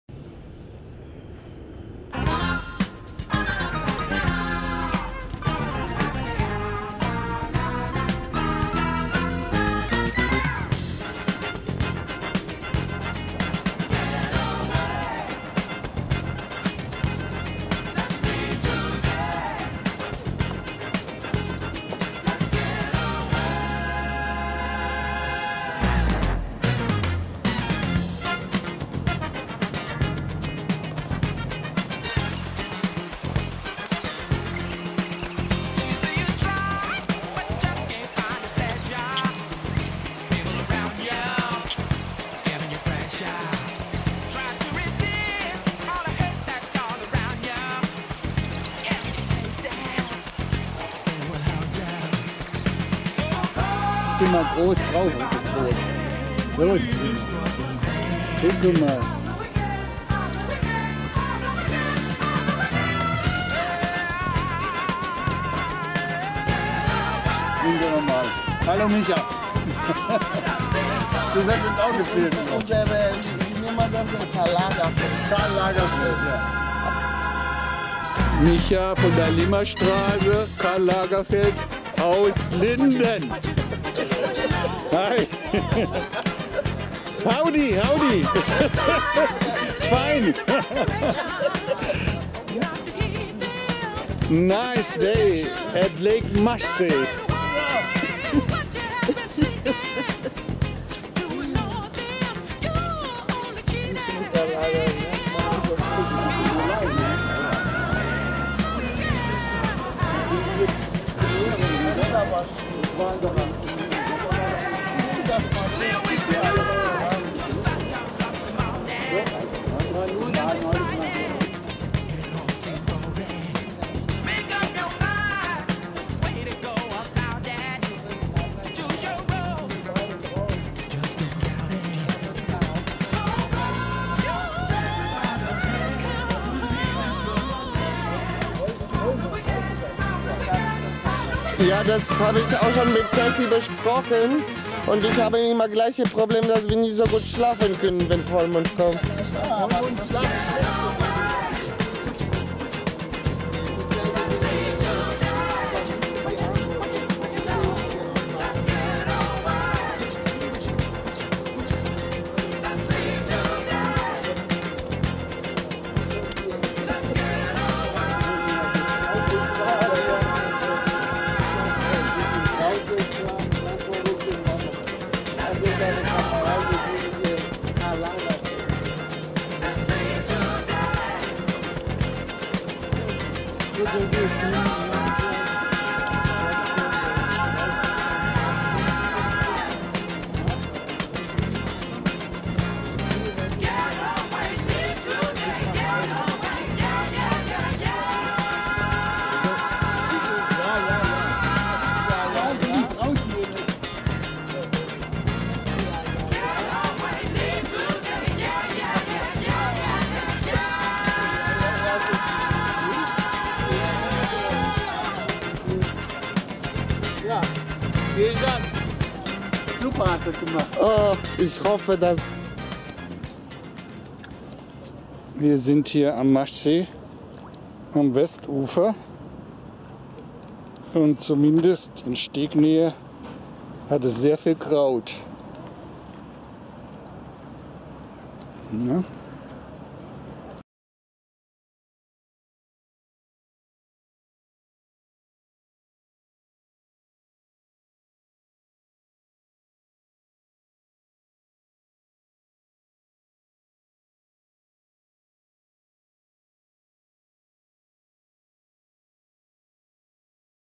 Int. Dragon 1m - weedless RC Sailboat (does not catch any weeds)
Int. Drachen 1m - krautfreies RC Segelboot (Vollkieler mit angehängtem Ruder)
H2o RC Design
- One Design Class
- Frees itself from weed
- Full keel with rudder does not catch any weeds